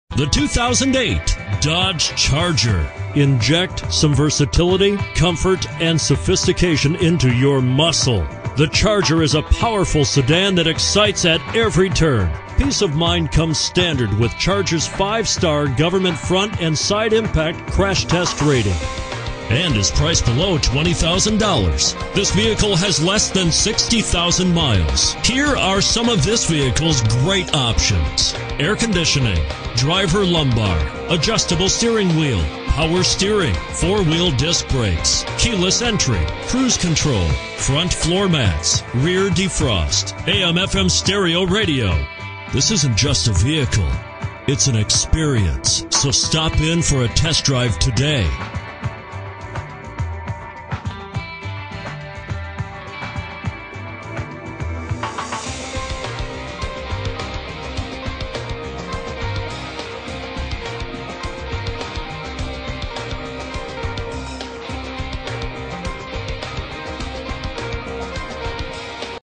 2008 DODGE CHARGER Cherry Hill, NJ 98604
2008 DODGE CHARGER Cherry Hill, NJ
Stock #98604

Pick up this Brilliant Black Crystal Prl 2008 Dodge Charger, available today. Featuring automatic transmission, it has 55411 miles. This could be the one you've been searching for.